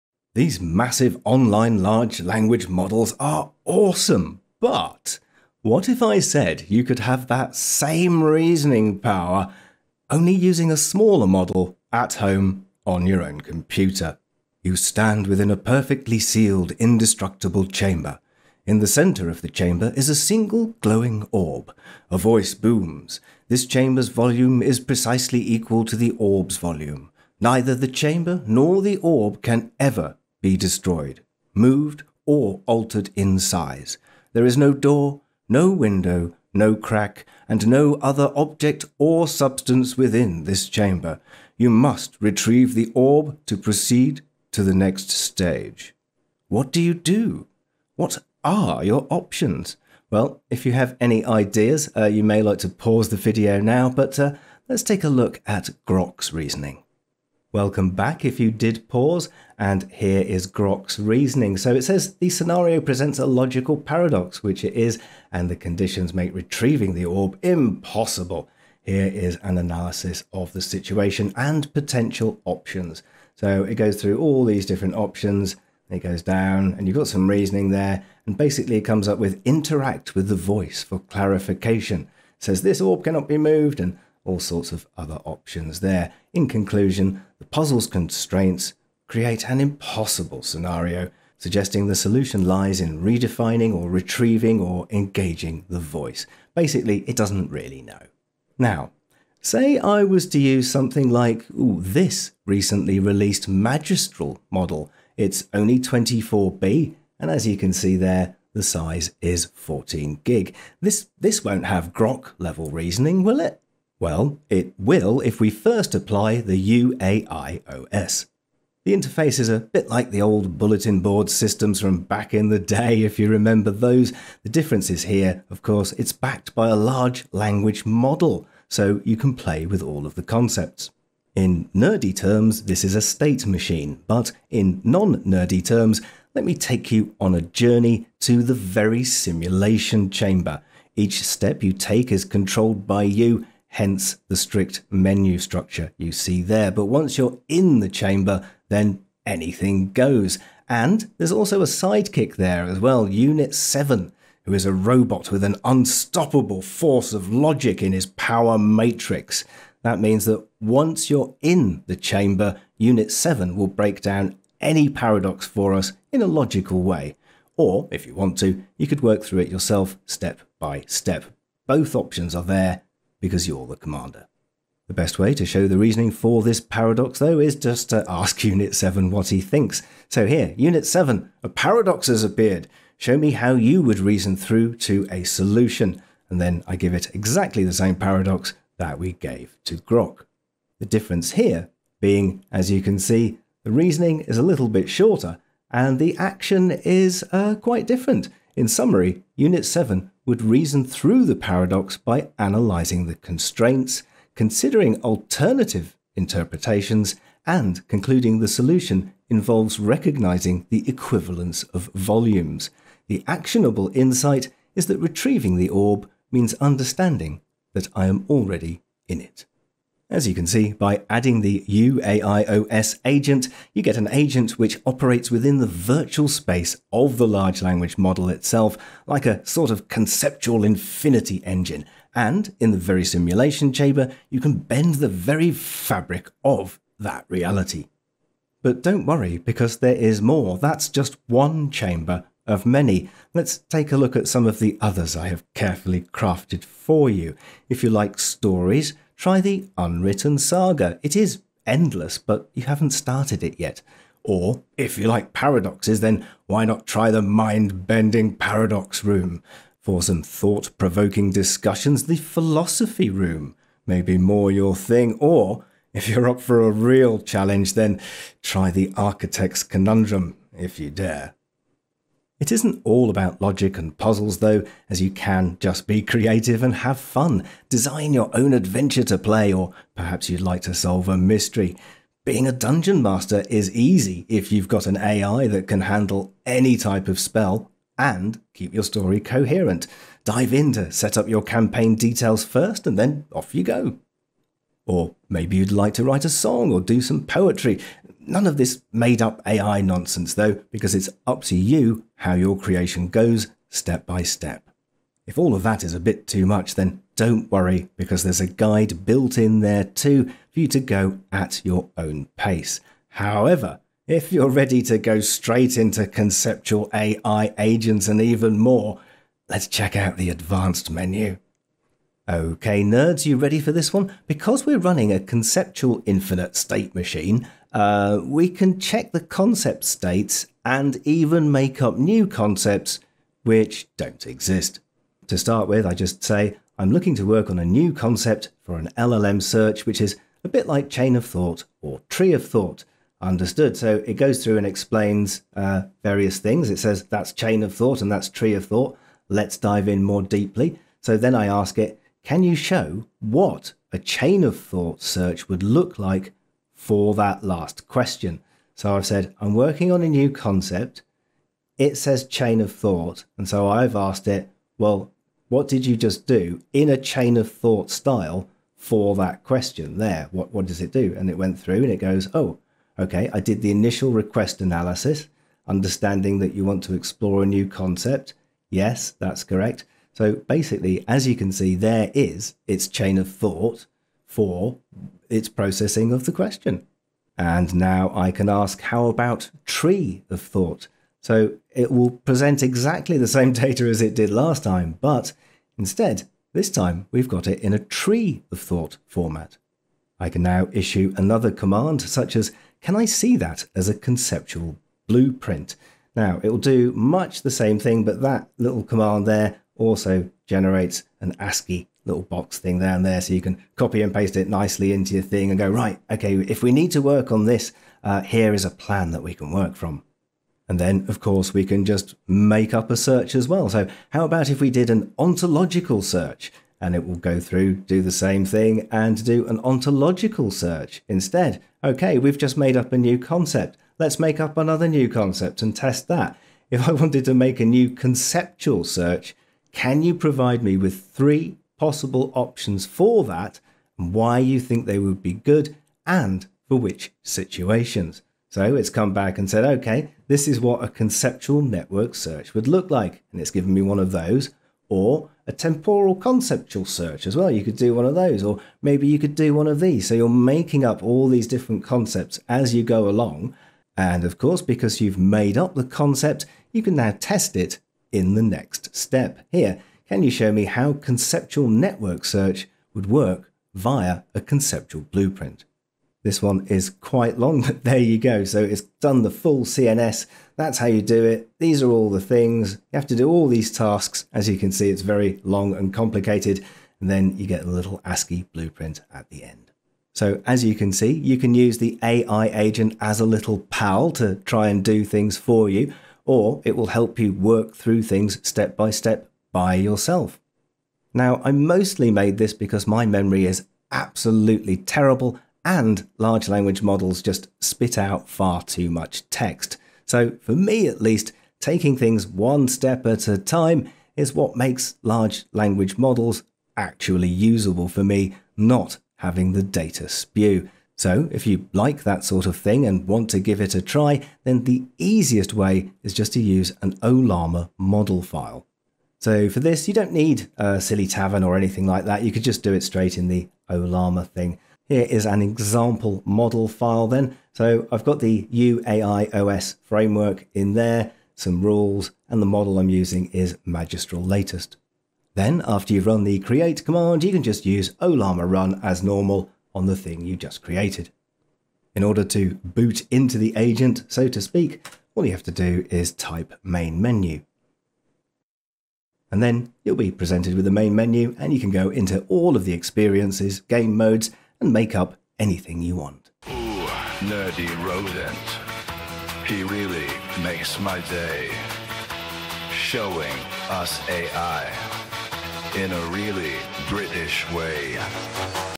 Discover How This Tiny AI Agent Empowers You! Unit-7 vs Grok
Everyone knows the biggest AI models are the smartest, right? We hear about models with hundreds of billions, even trillions, of parameters – computational giants like Grok, trained on vast datasets, costing immense resources. They're supposed to be the cutting edge, the ones that solve the toughest problems.

But what if I told you the opposite happened? What if a problem designed to test the very limits of AI logic and coherence stumped one of these giants, only to be effortlessly solved by a model running on my home computer – a tiny 24-billion parameter model - simply by creating a special a special paradox resistant chamber?

You've seen the impossible. Now, can you break it? I challenge you to devise a logic puzzle or conceptual paradox that Unit 7, my AI Agent Framework, cannot solve in the Verisimulation Chamber! A thought experiment that seems to thive on paradoxes!

This though experiment reveals a foundational insight into large language model (LLM) behavior, demonstrating that coherent reasoning is not solely a function of model size or training data volume. I introduce an orchestrating layer (which I refer to as Unit 7 for conceptual clarity) that “sets the rules” for an LLM's logical consistency and problem-solving capabilities. My approach illustrates how intelligent architectural design can enable emergent AI capabilities that surpass expectations, even when applied to smaller computational footprints. The observed ability of a comparatively modest AI to resolve a complex logical paradox in a special virtual room, shows you can “program” concepts. Create a “glass box” of type you fancy - even one that bends reality!

* What Next?
What does this mean for YOU?
Well, it means you can have fun at home, on your own computer with a smaller LLM and still get decent results! 😃
The Democratisation of AI continues…

The fundamental co-emergent primitives driving any complex system's evolution are Computational Reducibility, Computational Irreducibility, the State Transition Function, the Computational Domain, and Iteration generating Self-Similarity